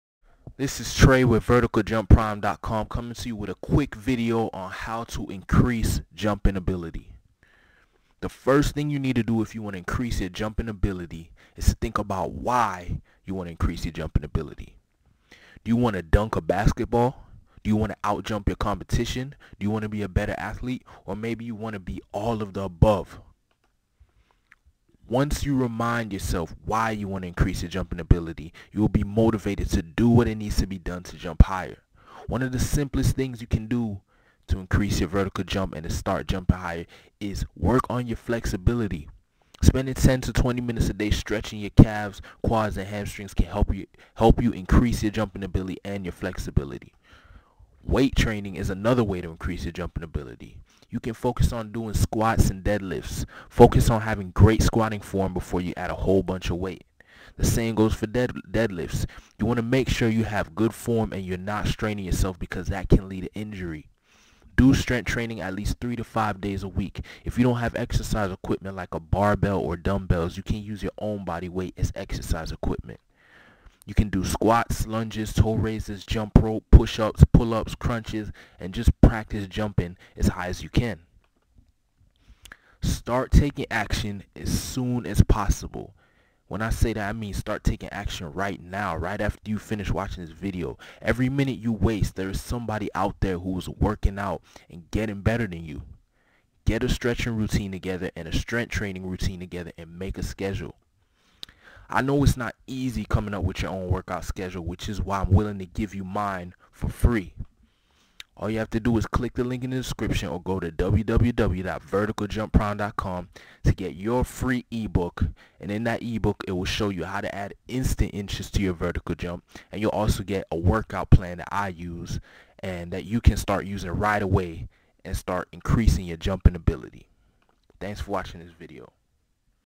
How To Increase Jumping Ability
Do you want to know how to increase your jumping ability? Well you can do that and it is actually very simple. People like to make things hard for some reason but when it comes to increasing your jumping ability there are just a few things you need to do. You have to increase your flexibility, your strength, and athleticism. That's it.

You don't have to pay hundreds or even thousands of dollars for a vertical jump training coach or anything like that. You can do exercises in the comfort of your home and literally add 10 inches or more to your vertical jump if you know what you're doing. Nothing happens overnight but if you take action and don't give up you will increase your jumping ability.